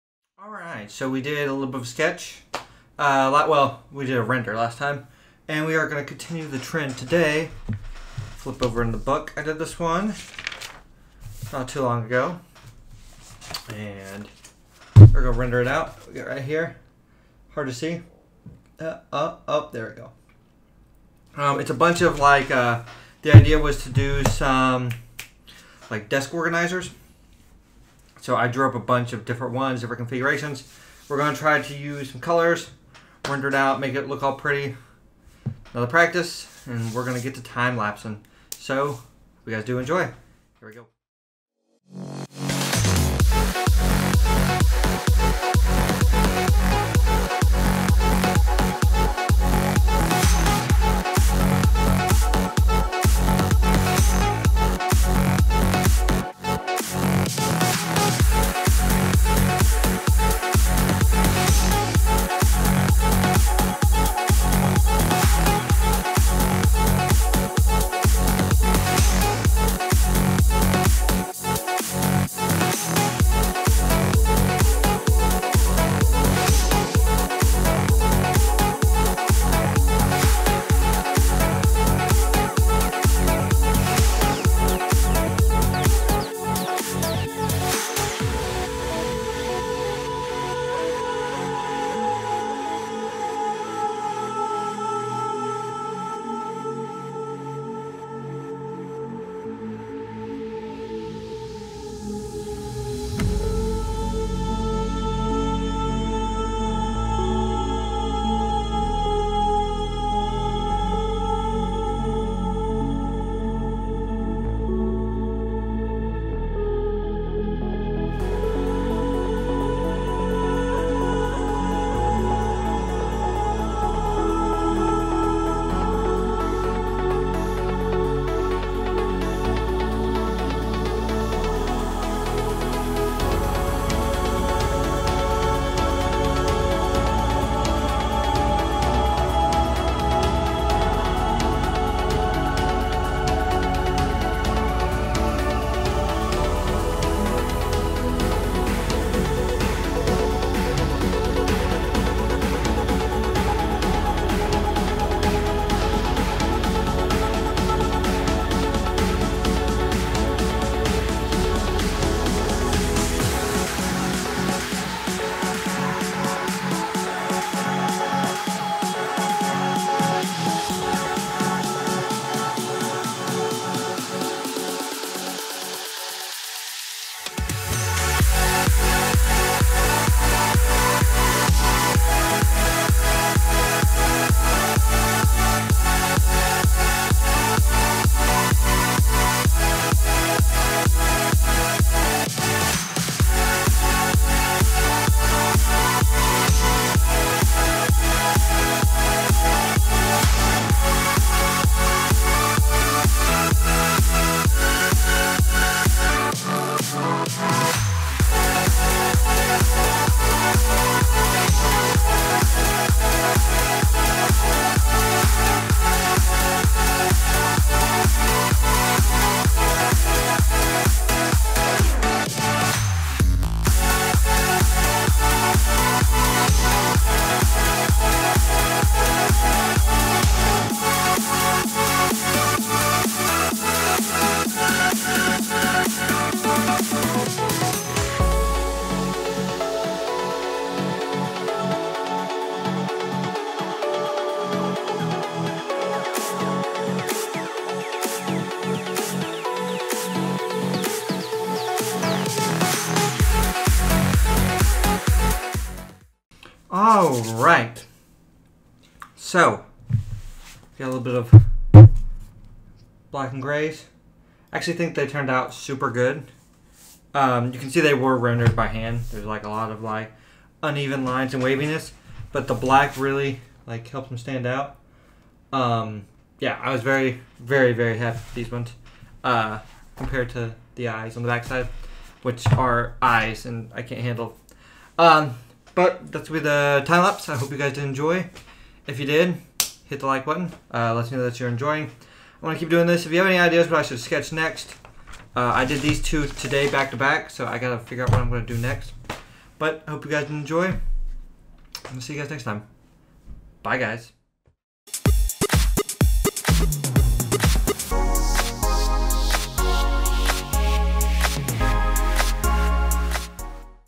Rendering Eps. 2: Office Organizer
Rendering out some office organizers that I sketched up.